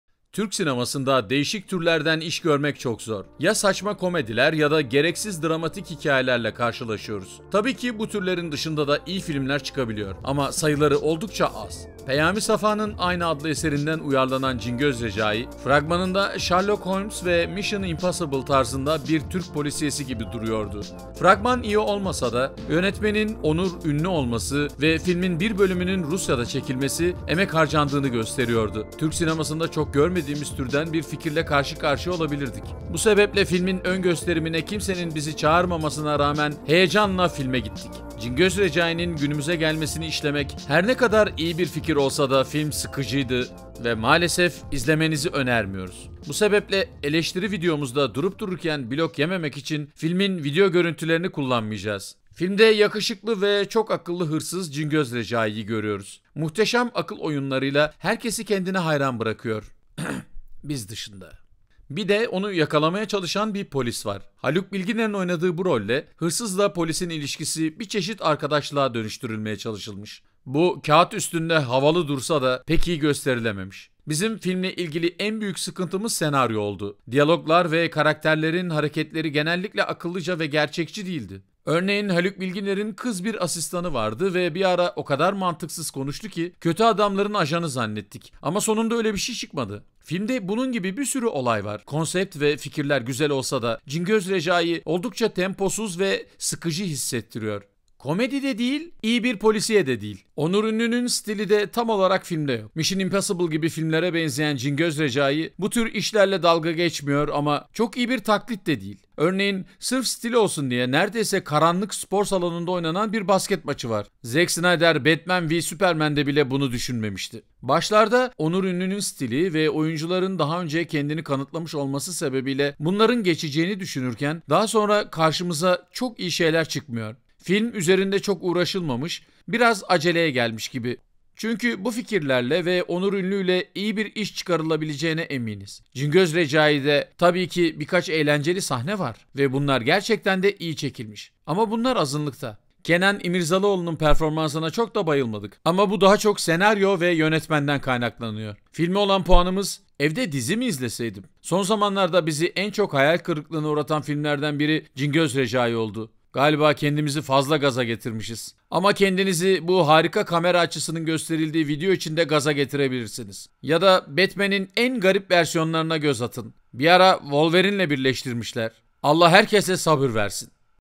Kanka Siz Naptınız? Cingöz Recai | İnceleme (2017)
Kenan İmirzalıoğlu ve Meryem Uzerli'nin başrollerinde yer aldığı ve Onur Ünlü'nün yönettiği Cingöz Recai filmini inceledik!

Türk sinemasında değişik türlerden iş görmek çok zor. Ya saçma komediler ya da gereksiz dramatik hikayelerle karşılaşıyoruz. Tabii ki bu türlerin dışında da iyi filmler çıkabiliyor ama sayıları oldukça az.

Peyami Safa'nın aynı adlı eserinden uyarlanan Cingöz Recai, fragmanında Sherlock Holmes ve Mision Impossible tarzında bir Türk polisiyesi gibi duruyordu. Fragman iyi olmasa da yönetmenin Onur Ünlü olması ve filmin bir bölümünün Rusya'da çekilmesi emek harcandığını gösteriyordu. Türk sinemasında çok görmediğimiz türden bir fikirle karşı karşıya olabilirdik. Bu sebeple basın gösterimi davetiyesi almamamıza rağmen heyecanla filme gittik.

Cingöz Recai'nin günümüze gelmesini işlemek her ne iyi bir fikir olsa da film sıkıcıydı ve malesef izlemenizi önermiyoruz. Bu sebeple eleştiri videomuzda durup dururken blok yememek için filmin video görüntülerini kullanmayacağız.

Filmde yakışıklı ve çok akıllı hırsız Cingöz Recai'yi görüyoruz. Muhteşem akıl oyunlarıyla herkesi kendine hayran bırakıyor.